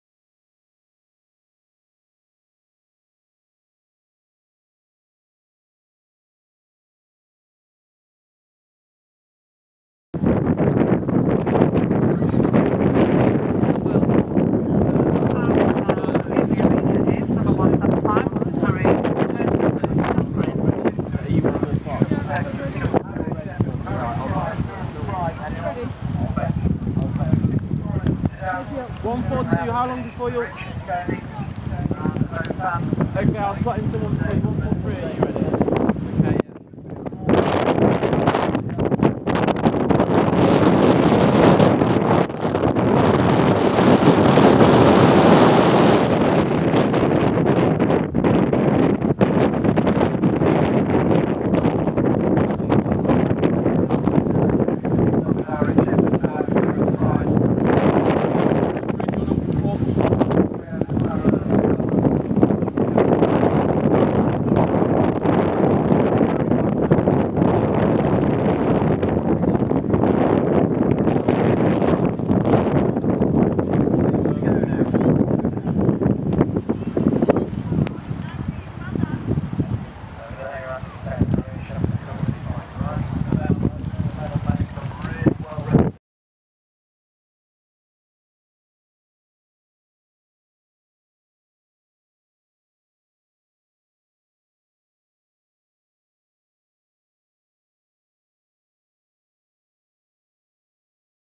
Harriet Turton & The Last Pilgrim: Norton Disney ODE Show Jumping
This is the Show Jumping section from the Pony Club ODE at Norton Disney. Clear Round. For a full report, check out and follow our progress throughout the season on Facebook: HTEventing and on Twitter: @HTEventing